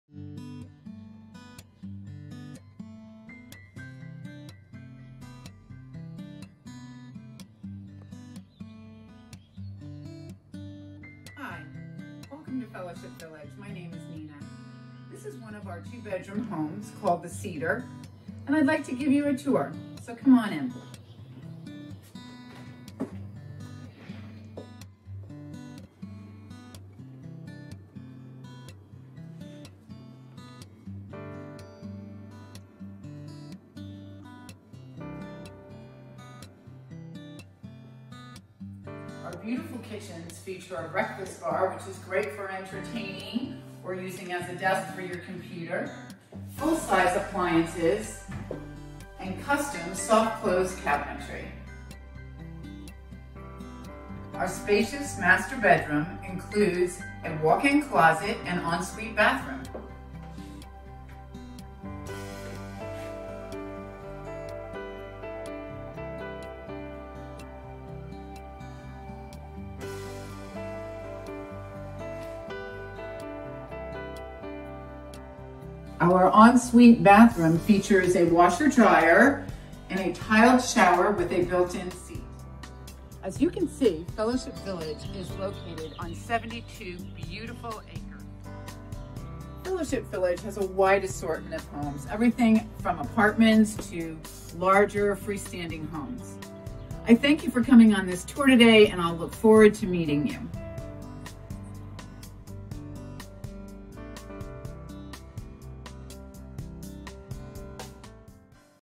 Cedar Home Virtual Tour | Senior Living in Basking Ridge, NJ | Fellowship Senior Living
Watch a virtual tour of Fellowship Senior Living's Two Bedroom Cedar Home. Located in Basking Ridge, NJ, our life plan community offers independent living, assisted living, memory care and more!